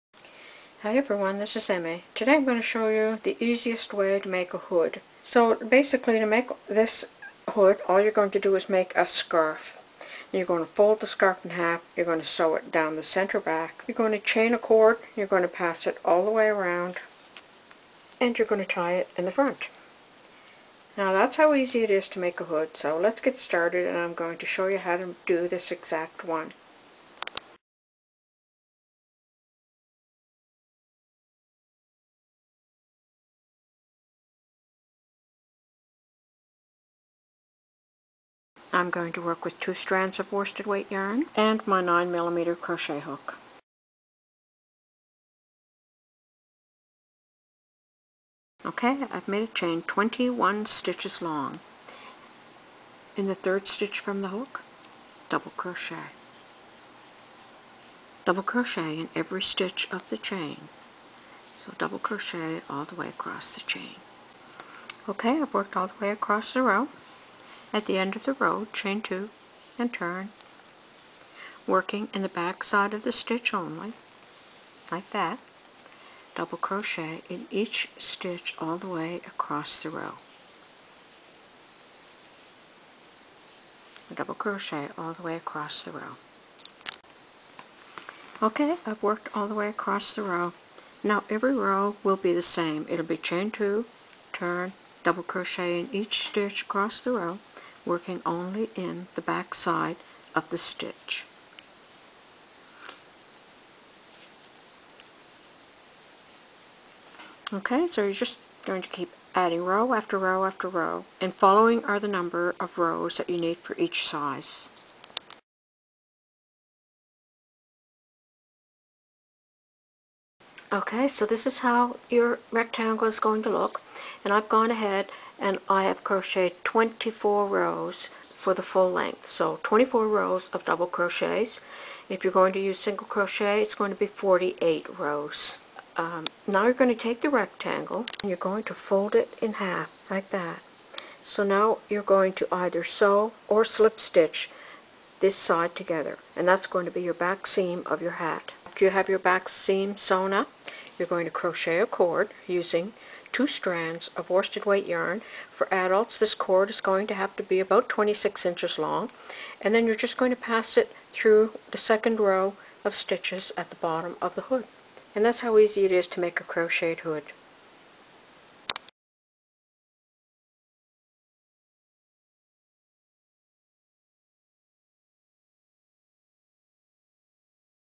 HOW TO CROCHET A HOOD, easy method, baby, toddler, child, teen, adult, make any size!
MY SHOPS:
(new Hectanooga shop with only hats and scarves)

(Knitting, and sewing patterns)
(sewing, crochet, knitting patterns)